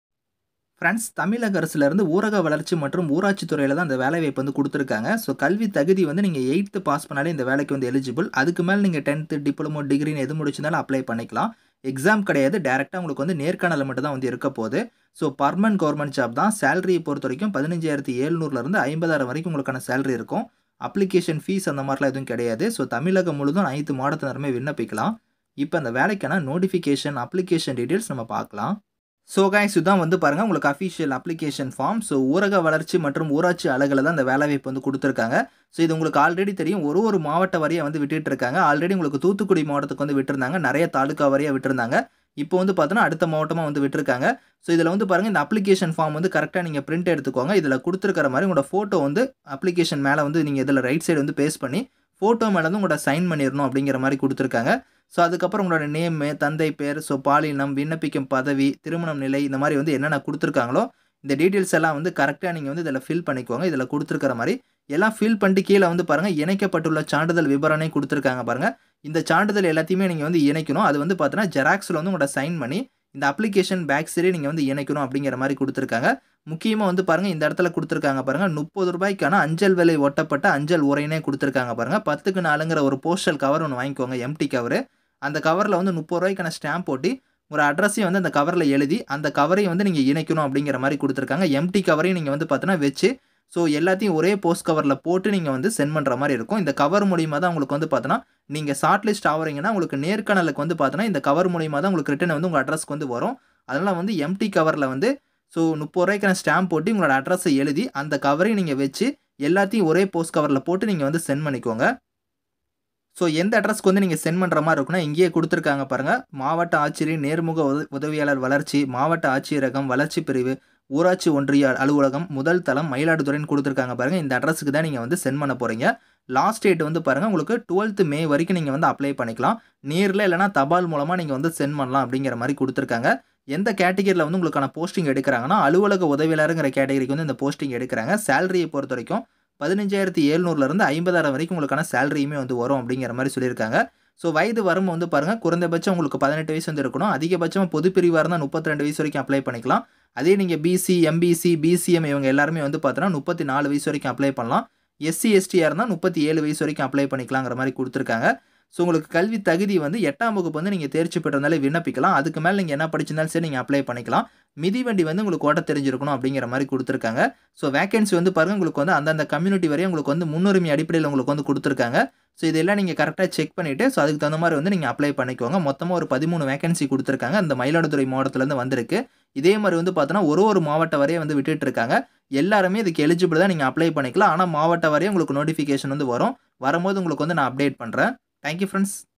🔥ஊரக வளர்ச்சி துறையில் வேலை//சம்பளம்-15700+படிகள்//8th Pass//தேர்வு இல்லை//Government Job//TAMIL..
தமிழ்நாடு அரசு ஊரக வளர்ச்சி துறையில் வேலை..